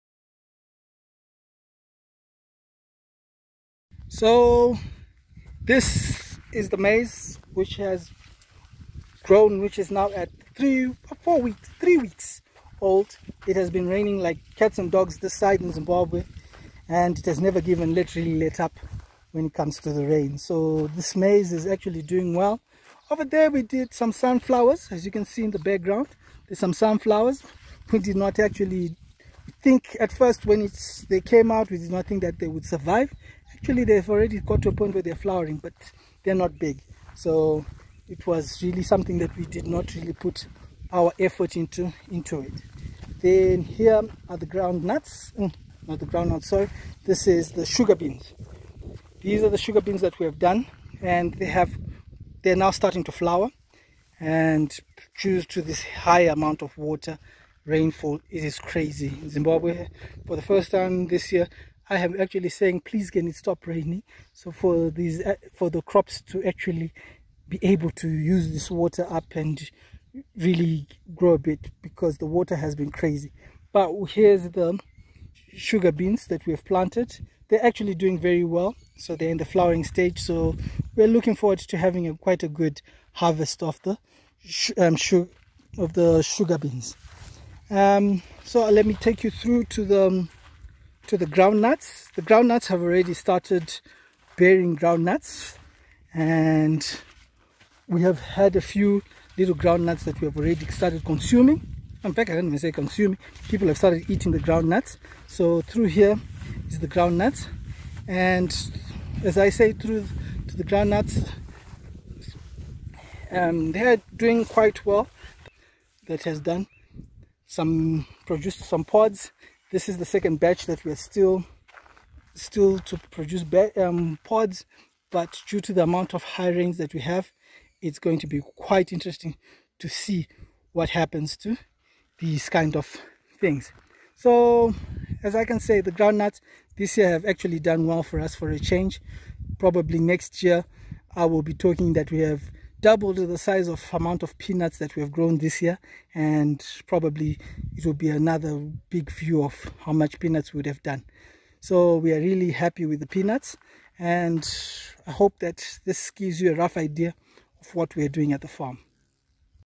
Week 7 Maize & Sugabeans & Ground nuts Update
At week 7.
The rains have not let up with more rains expected in the month of February 2021
The additional maize that we planted mid-January 2021 is doing well
The Sugar beans that we planted 3 weeks ago has started to flower
While the groundnuts are doing well